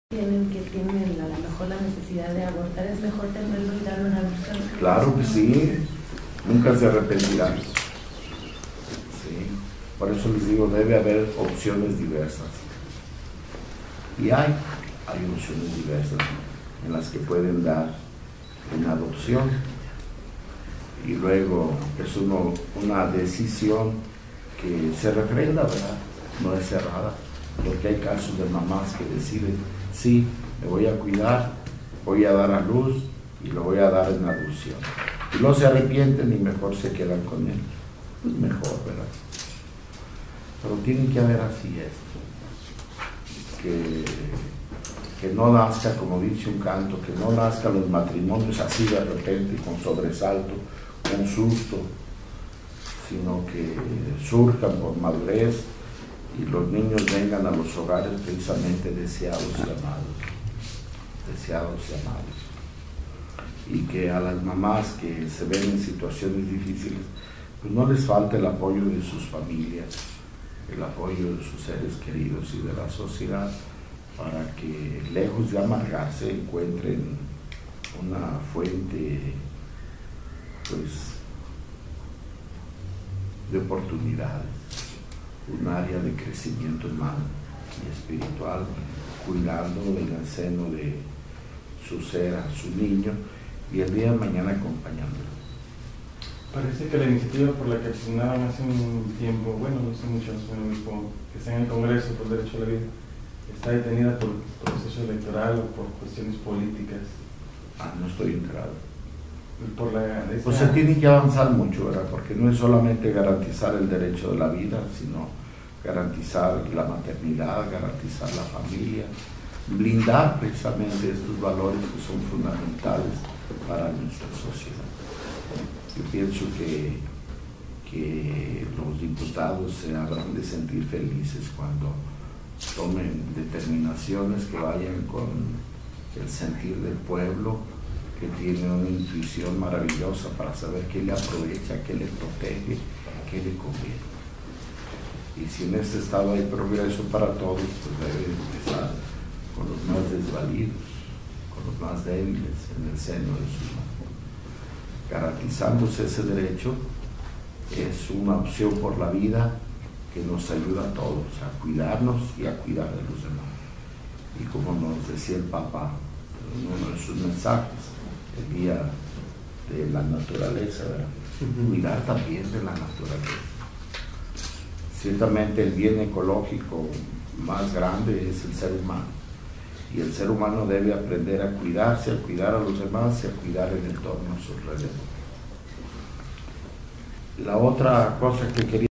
DIÓCESIS AGS.MÉX. 2 QUÉ NO NASCAN LOS MATRIMONIOS DE UN SOBRESALTO 06MAY2013
QUE SURGAN POR MADUREZ Y QUE LOS NIÑOS LLEGEN DESEADOS.
COMENTA EL SR. OBISPO JOSÉ MARÍA DE LA TORRE MARTÍN.